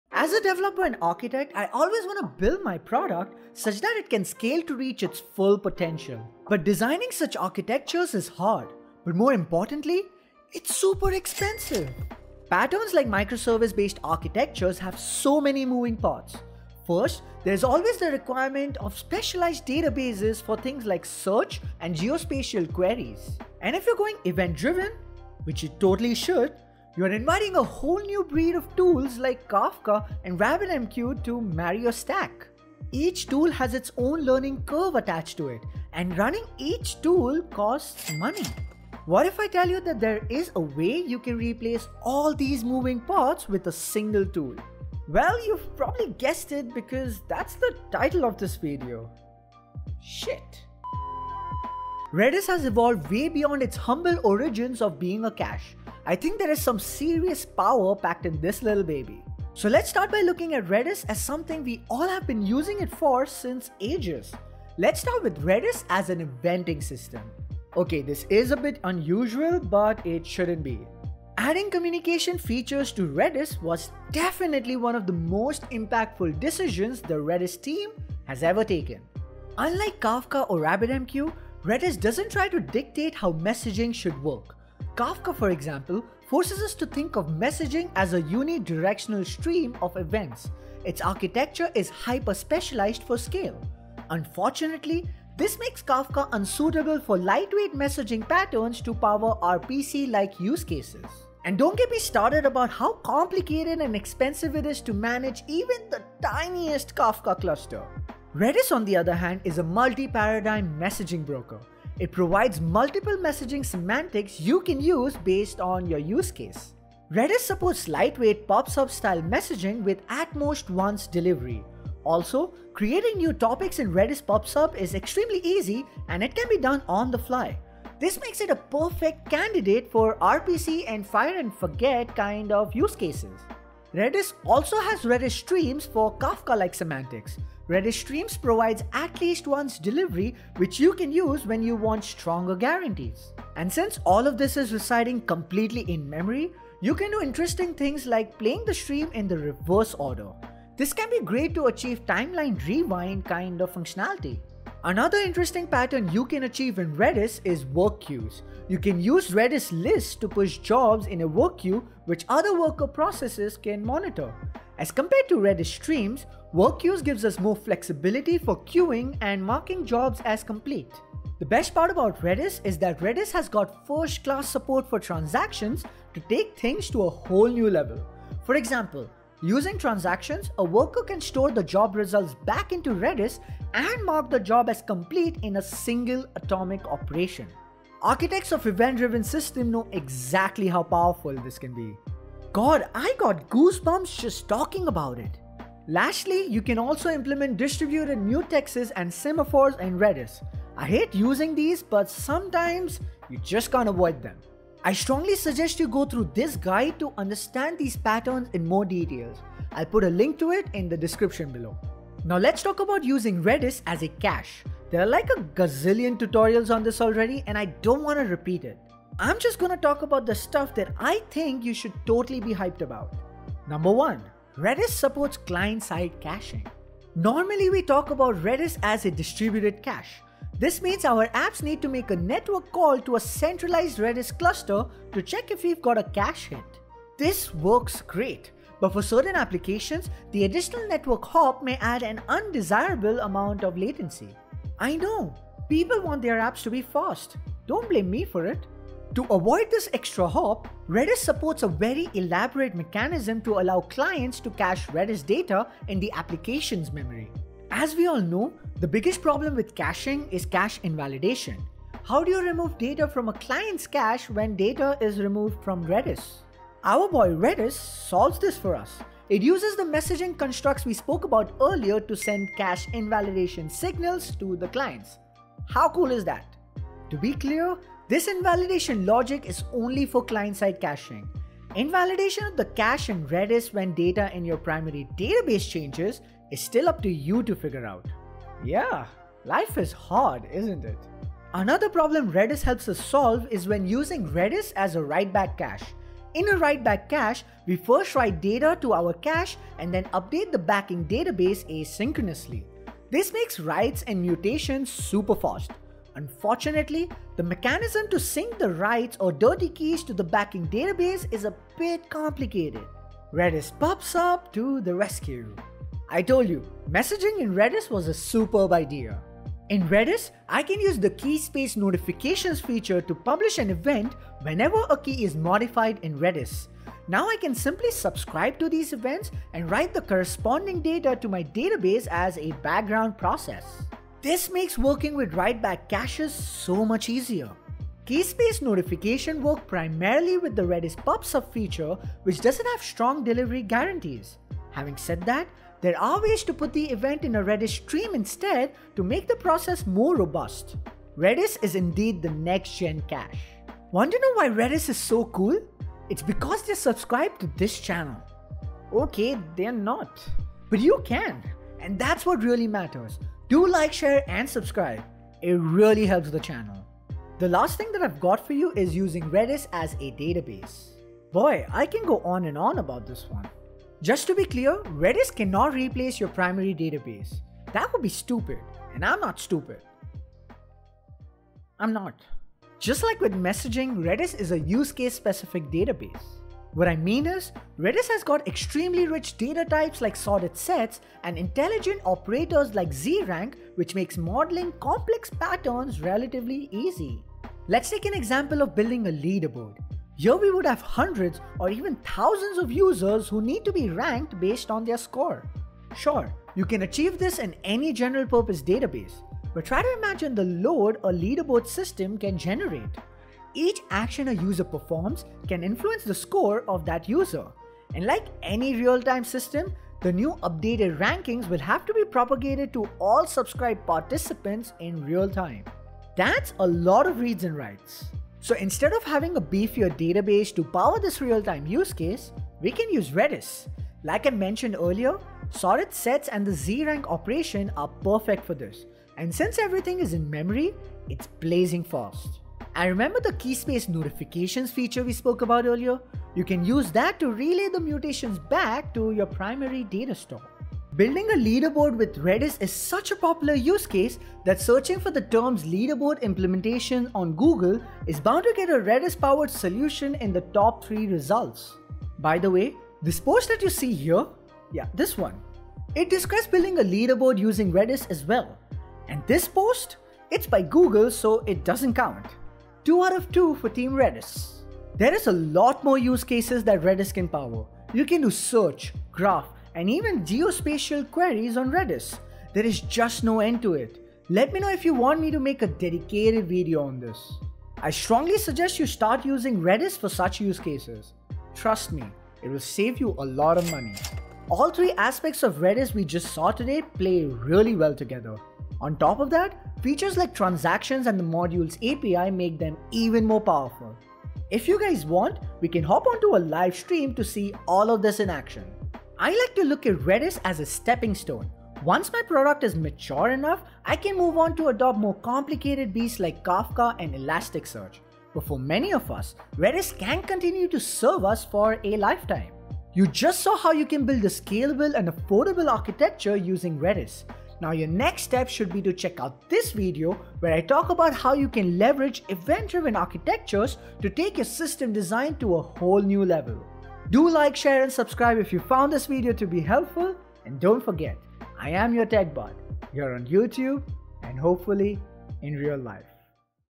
How REDIS Can Save You a Ton Of MONEY?!
Redis is perhaps the most underutilized tool! Here's how you can leverage the capabilities Redis has to offer to take your Microservices to your next level.

In this video we will look at:
1] How Redis can be used as the core of an eventing system
2] The next-gen caching capabilities Redis has to offer.
3] How you can use Redis as a specialized database.

Chapters
00:00 - Introduction
00:58 - Flexibility Eventing System
03:31 - Next Generation Cache
06:06 - Hyper Specialized Database
08:07 - Wrapping Up